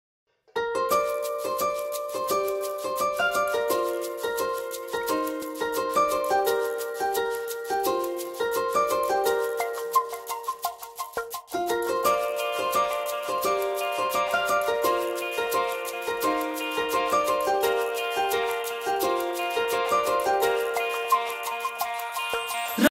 Mix Instrument
Childran playing mix instrument musical